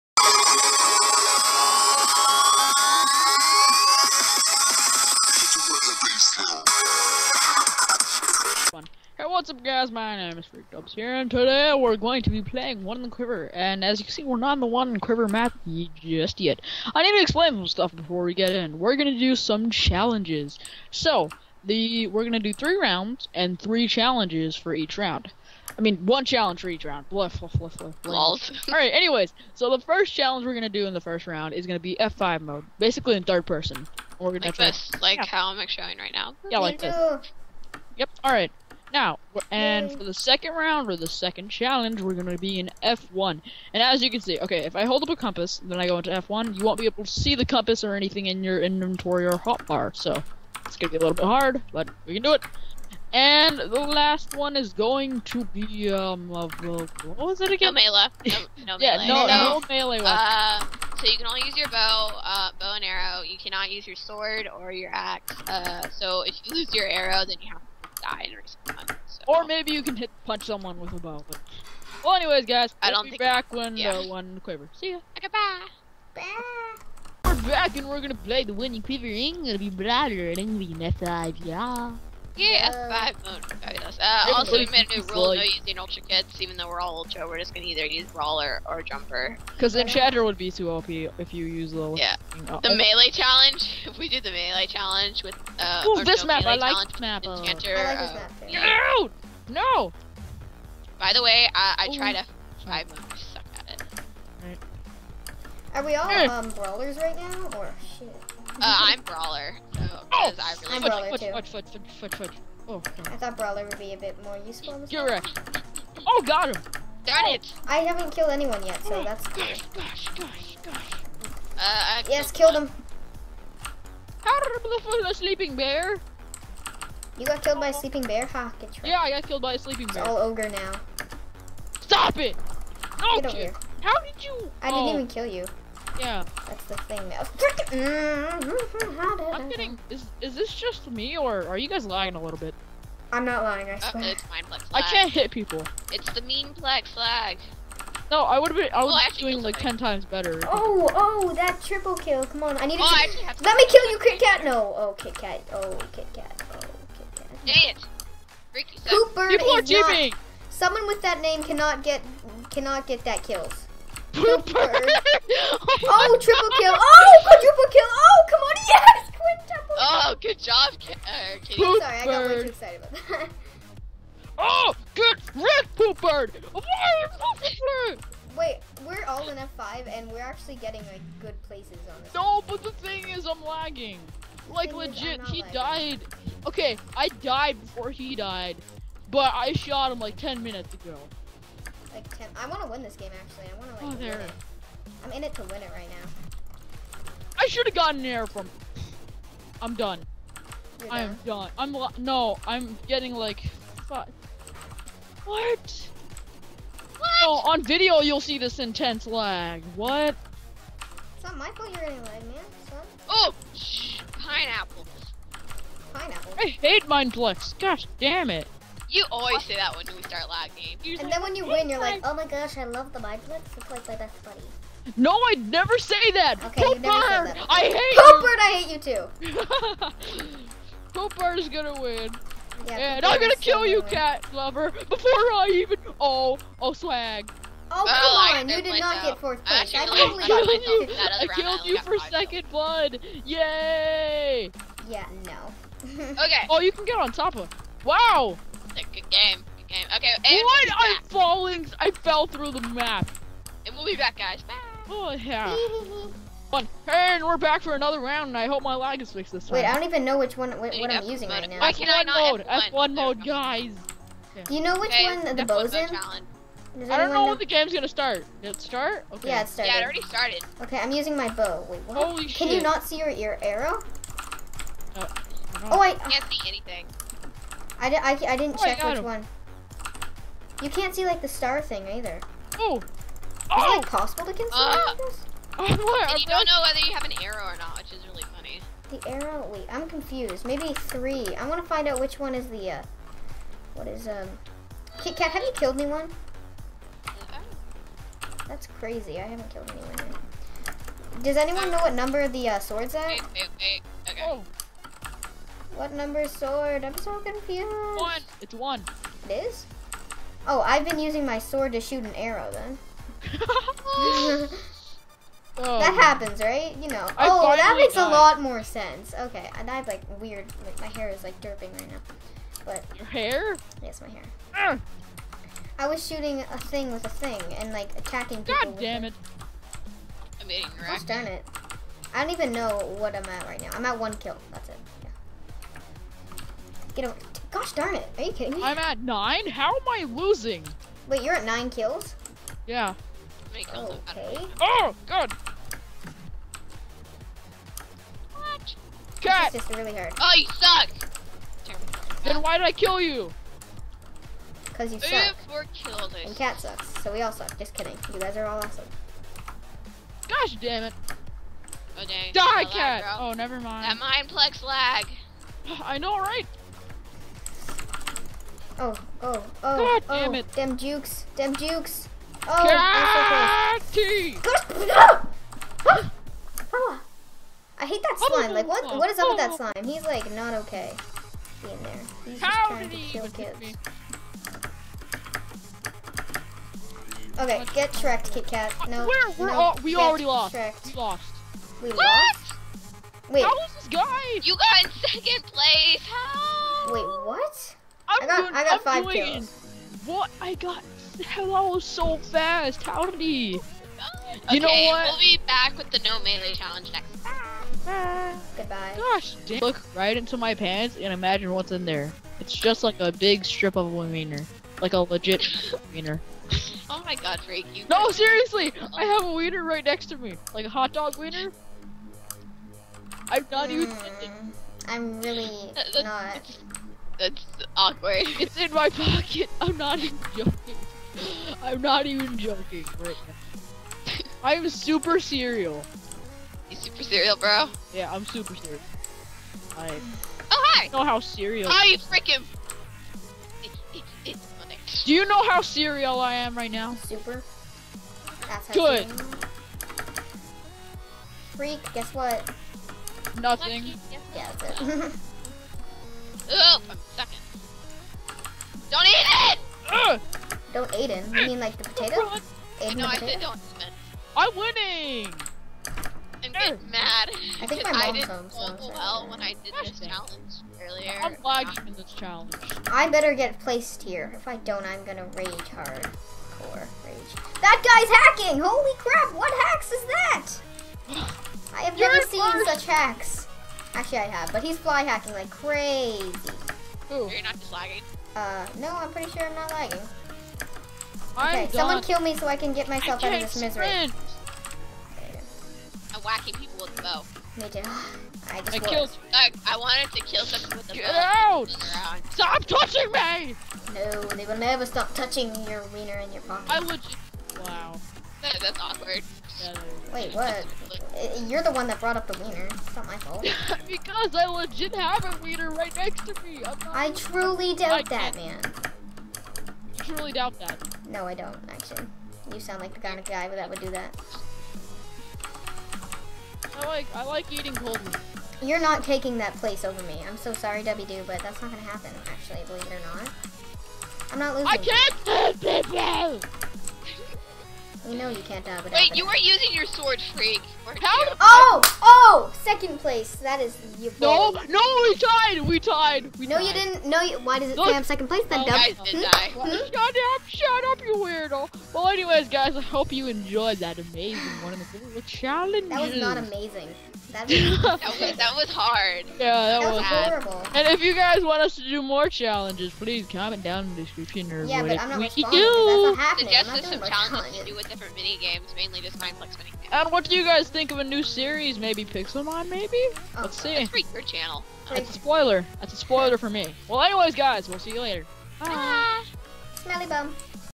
One In the Quiver Challenges With Friends
I hope you guys enjoy this awesome minigame challenge video with Dubby doo and Kat! Leave in the comments down below if i should do more of these with friends or solo!

These Amzing People